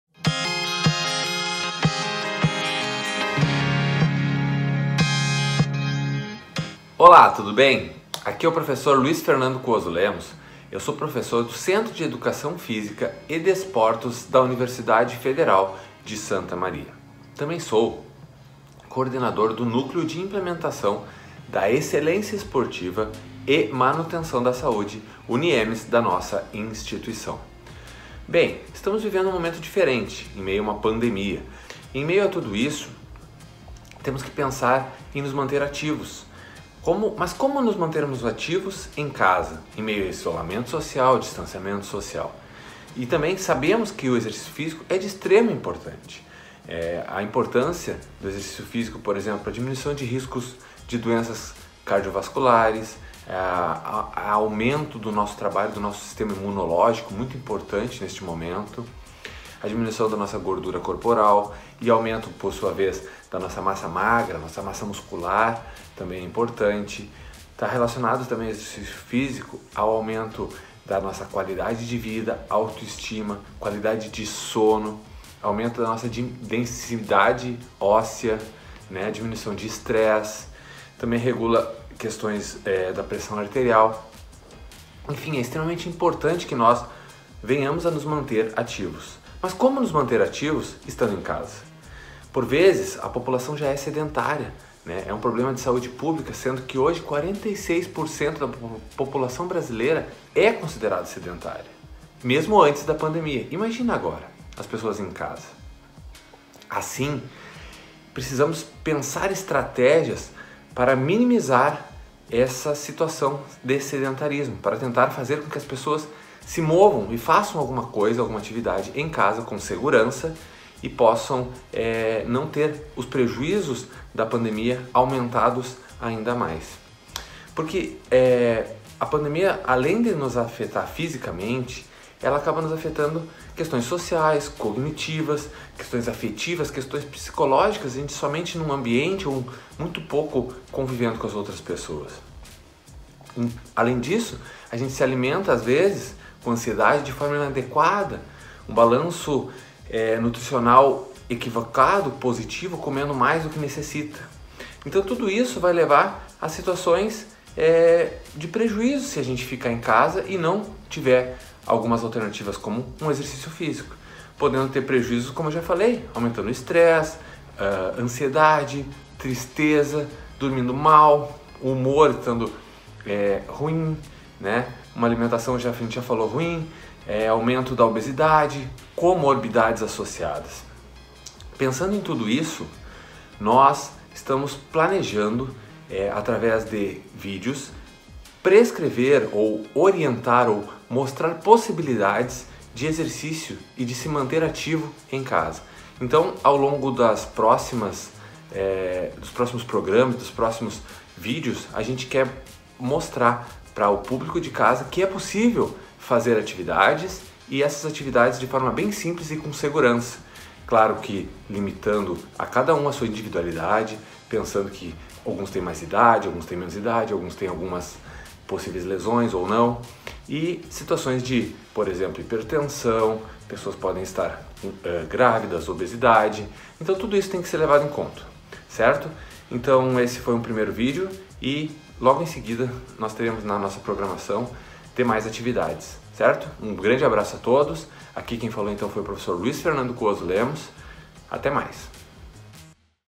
Importância de atividades físicas na quarentena
Diminuição do risco de doenças cardiovasculares e reforço do sistema imunológico estão entre os benefícios de se realizar atividades físicas durante a quarentena, mesmo estando em casa. O professor Luiz Fernando Cuozzo Lemos, coordenador do Núcleo de Implementação da Excelência Esportiva e Manutenção da Saúde - NIEEMS da UFSM, vai colaborar com a TV Campus para trazer dicas de como se mexer em casa.